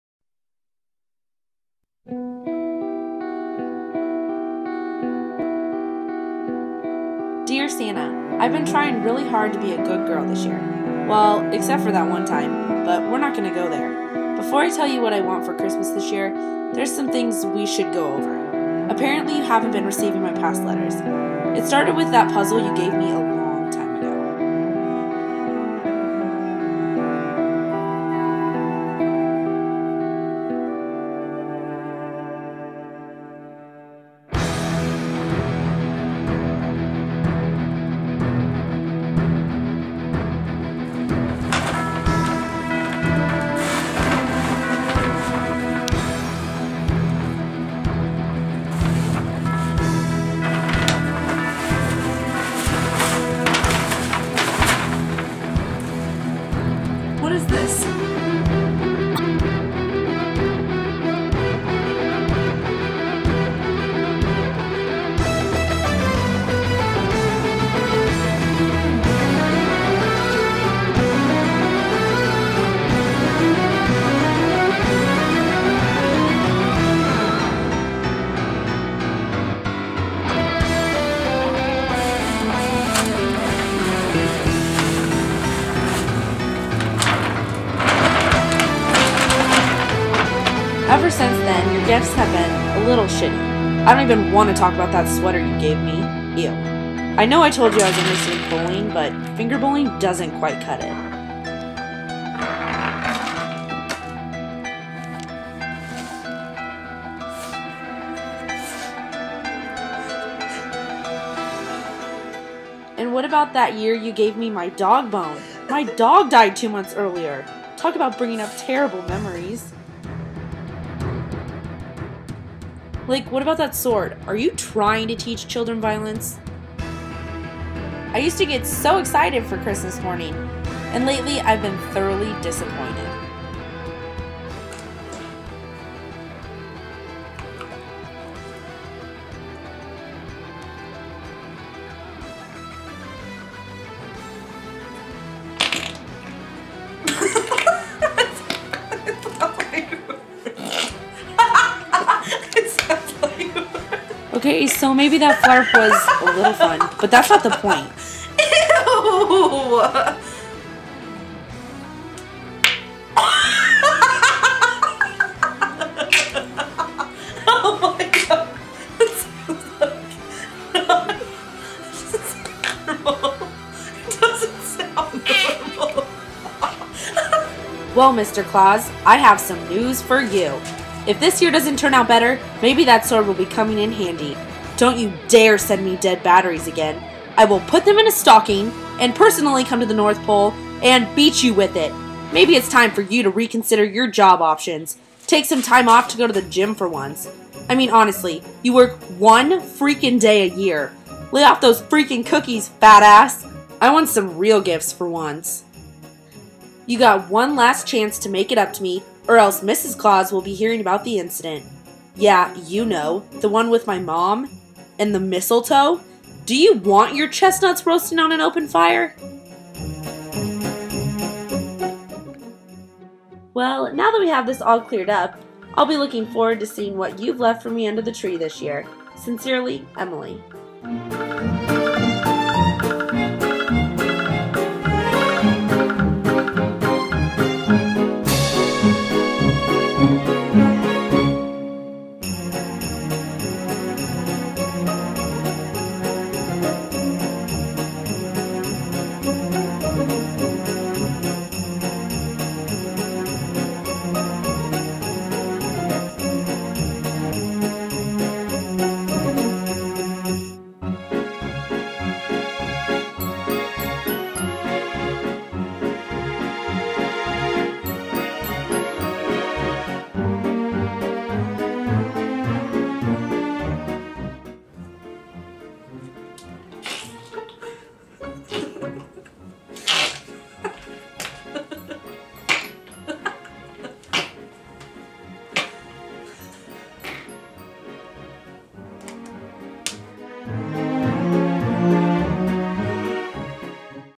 Unhappy Holidays
A rather unhappy girl decides to take up her yule tide frustrations with the big man in red...